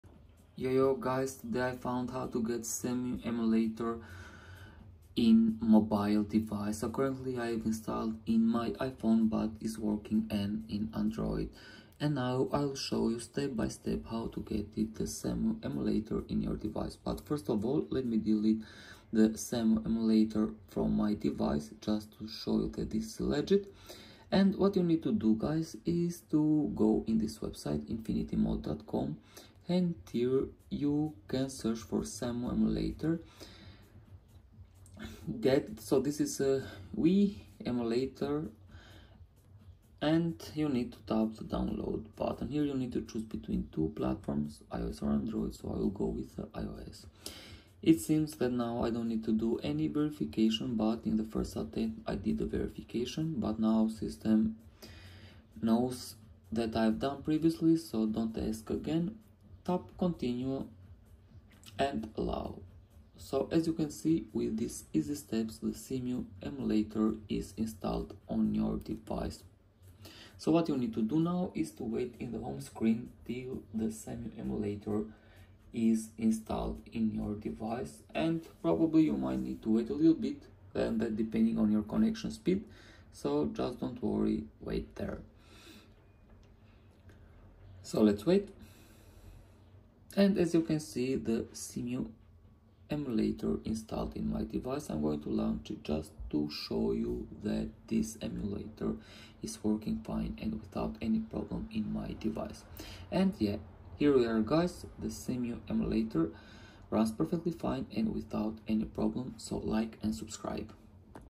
Cemu Emulator iOS iPhone iPad (2025)
hi guys, here u can learn how to do Cemu Emulator . works for ios android